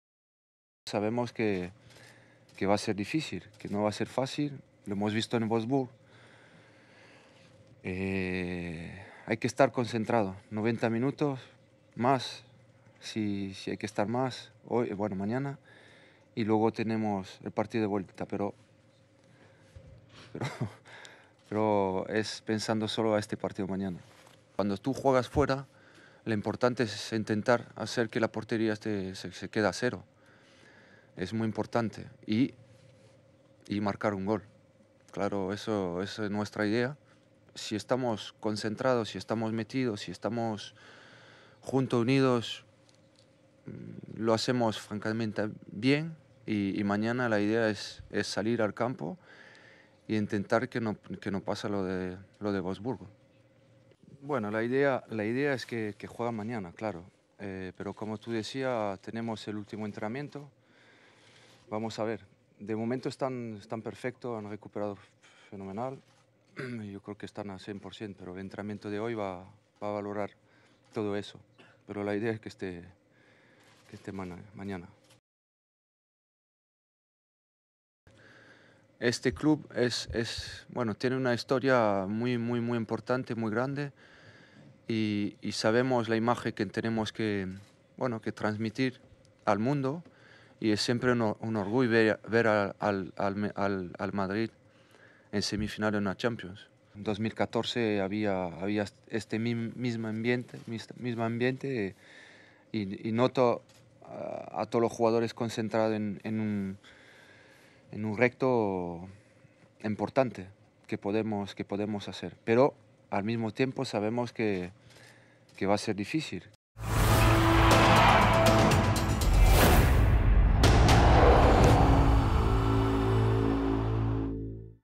Zidane: "Lo importante es dar el 150%"
El técnico aseguró, en la sala de prensa del Etihad Stadium, que "en el equipo que ganó la Décima había este mismo ambiente”.